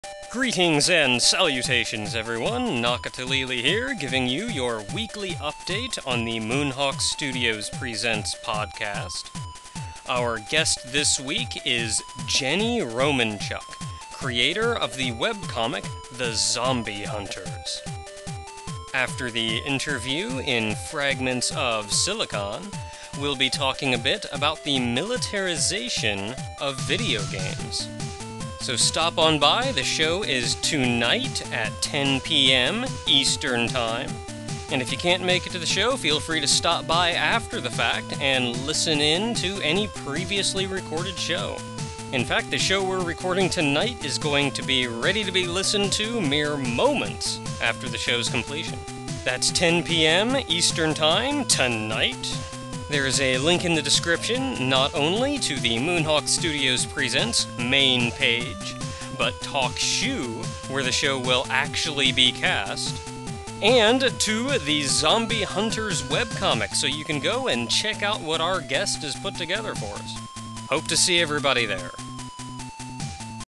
Moonhawk Studios Presents [p.10]
Moonhawk Studios Presents interview, Wednesday evening, the 13th (tonight), at 10pm EST, ElementalOgre and myself will especially be there for the later part of the show, Fragments of Silicon. The guest this week is Jenny Romanchuck of The Zombie Hunters